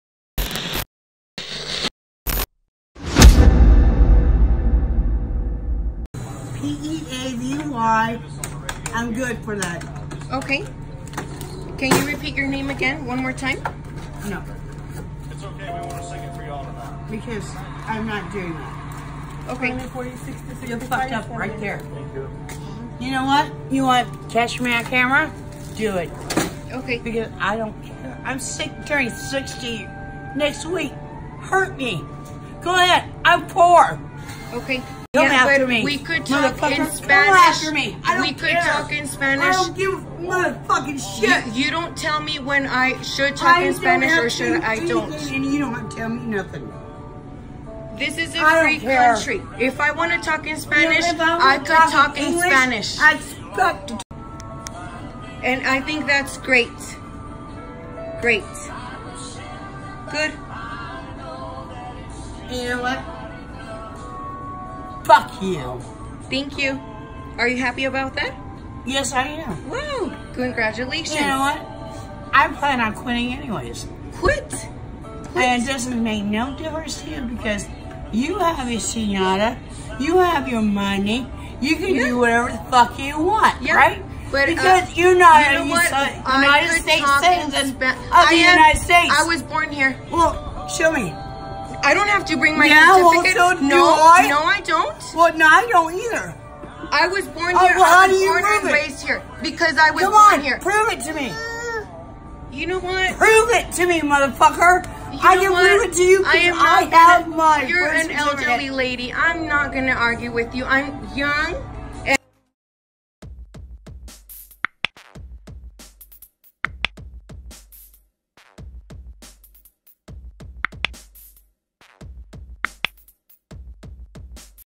Gas Station Cashier Screams Obscenities at Customer for Speaking Spanish || GoViral
Police say the victim ( Grecya Moran ) was speaking Spanish to a clerk when another employee intervened and told them to speak English.

Grecya Moran: "This was a horrible experience that i had to live with my son in my arms."

Grecya Moran was abused by a cashier after allegedly greeting another Shell staff member in Spanish.